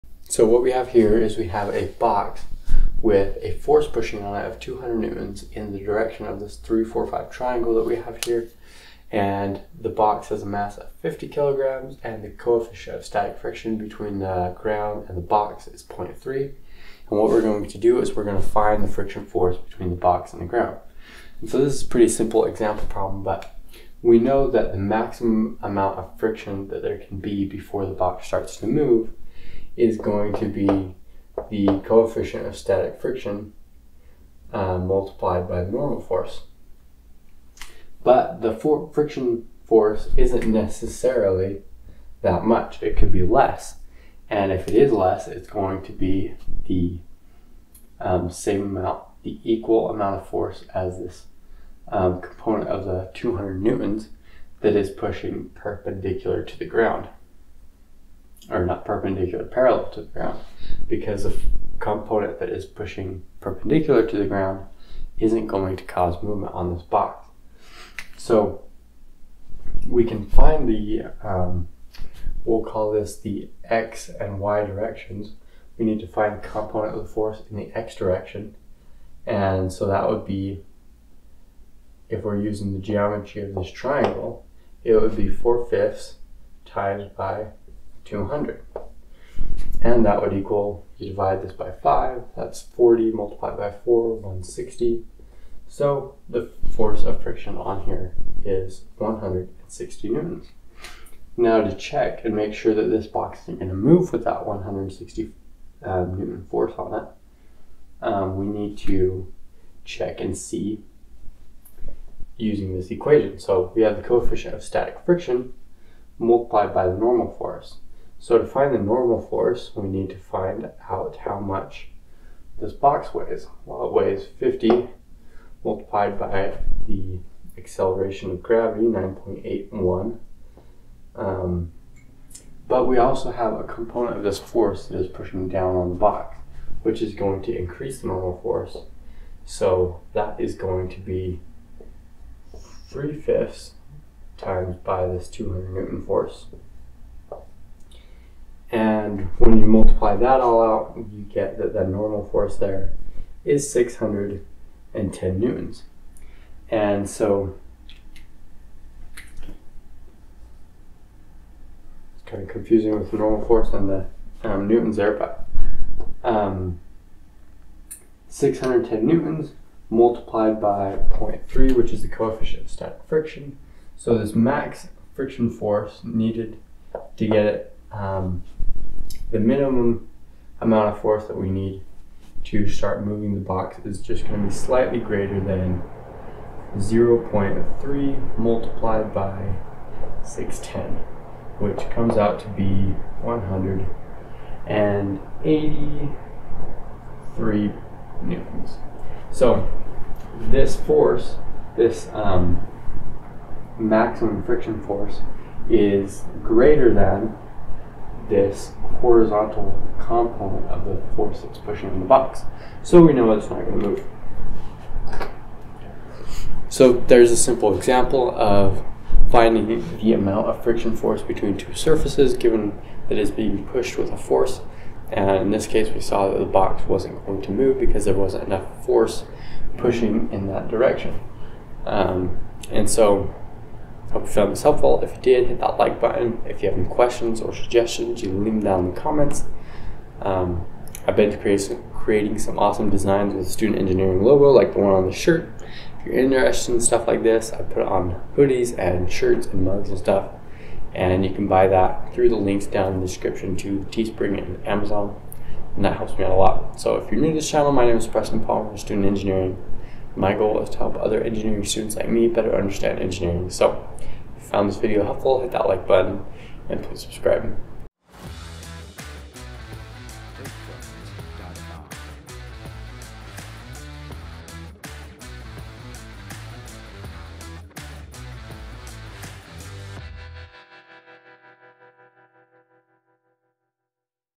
SIMPLE FRICTION EXAMPLE PROBLEM // Friction Explained // Equation and Explanation Given
In this video I go through a simple example problem involving friction. I explain the equation for friction and how it applies and how to solve for the force of friction.